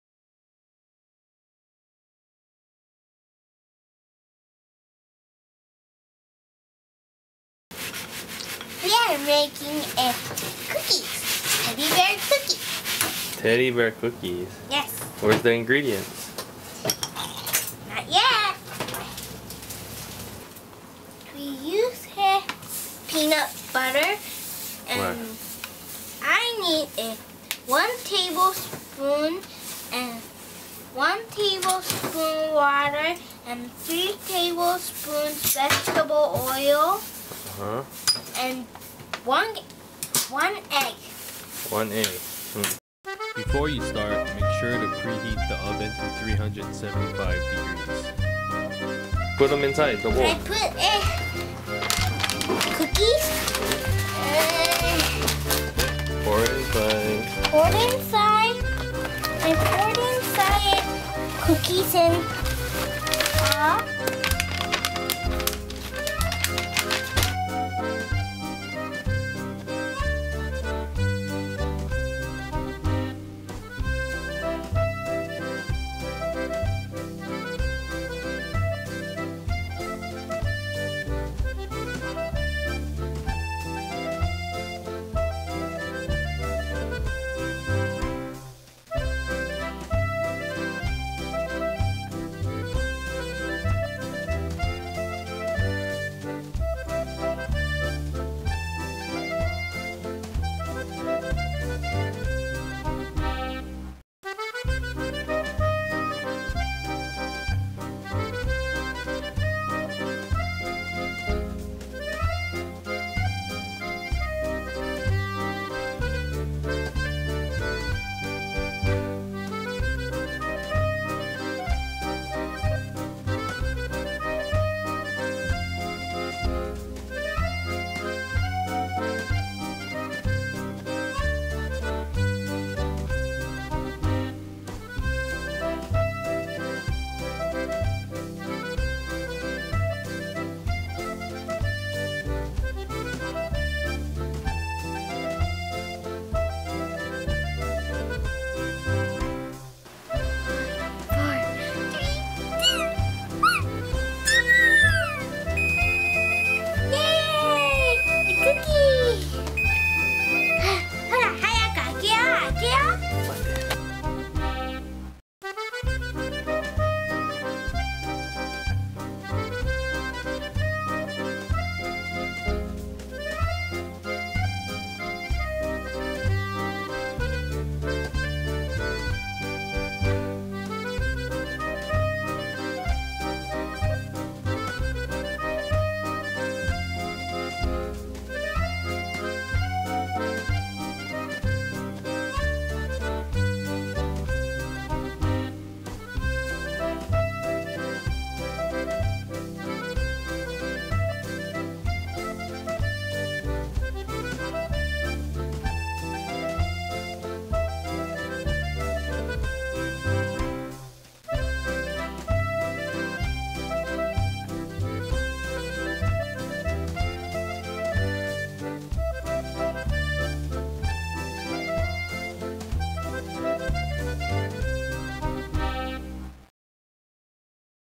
How to make cookies: Teddy bear cookies
my daughter wanted to make peanut butter teddy bear cookies so, we decided to make some and video it while we made them. enjoy the video and i hope you learn how to make teddy bear cookies too. aloha